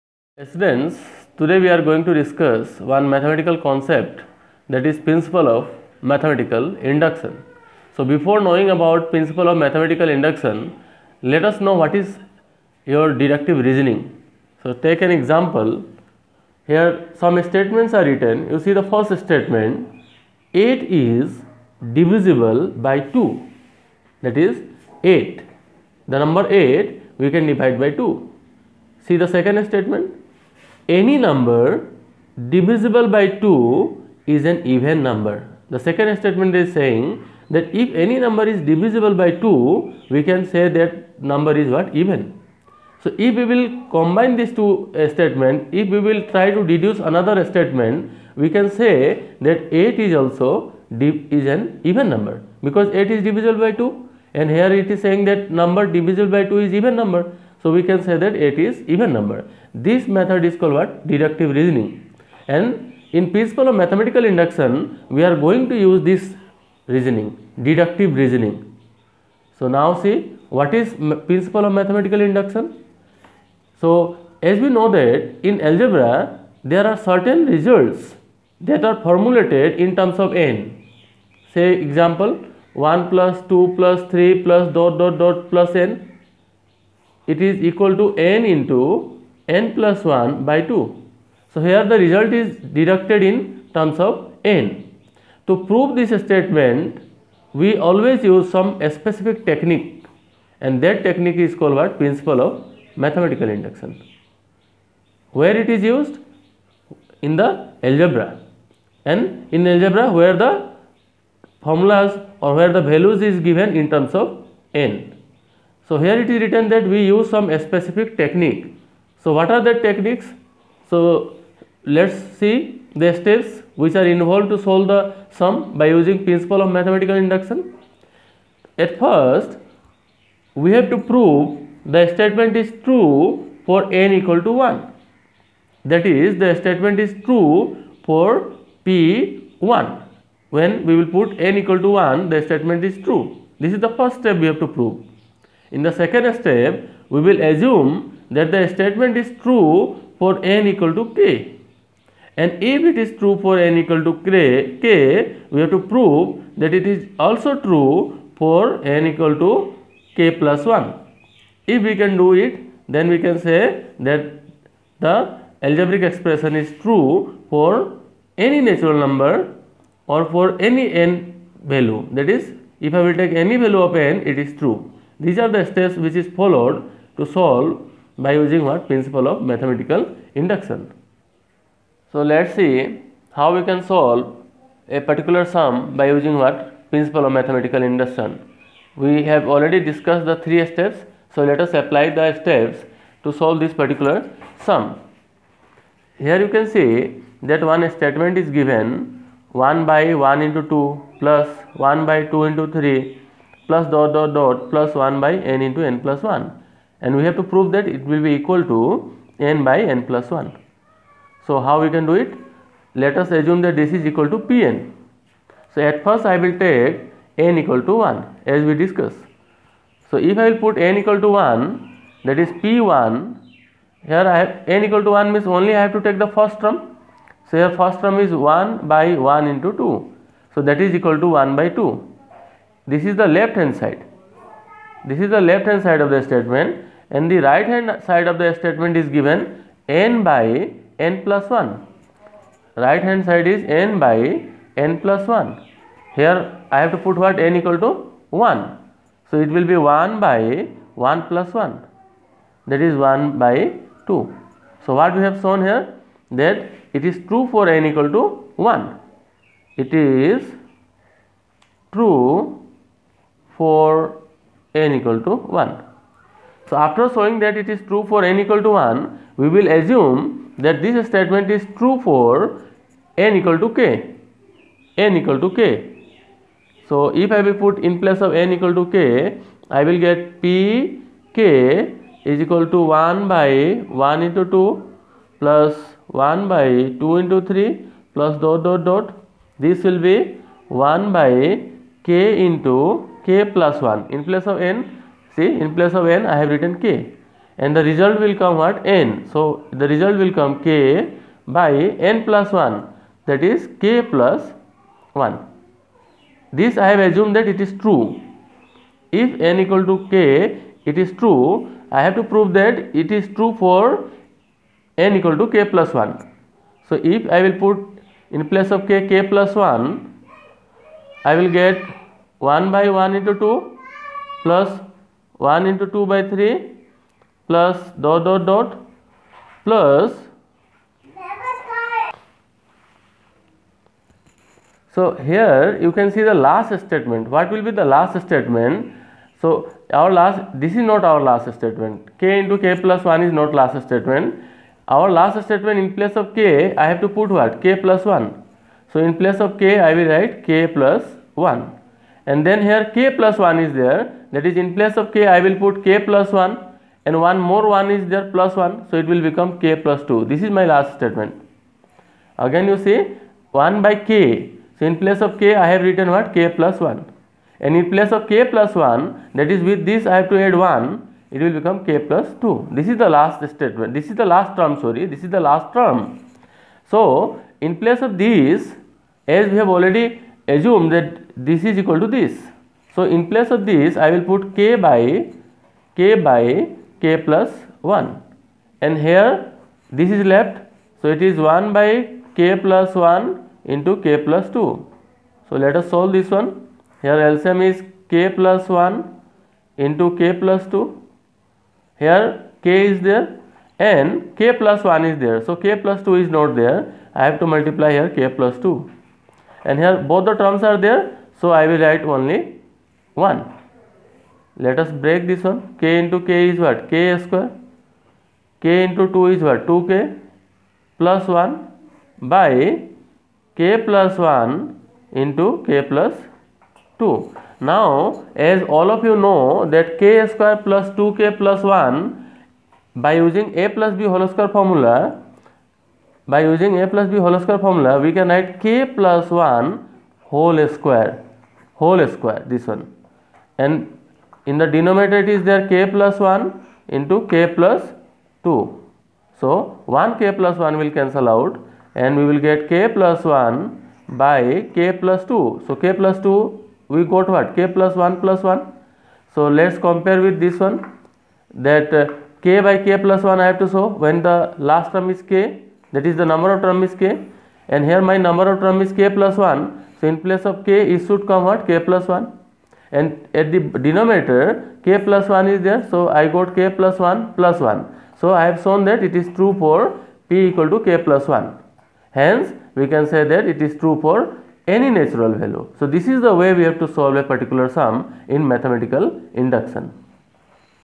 Mathematical induction
Discussion on mathematical induction with examples and steps required to follow while doing numericals.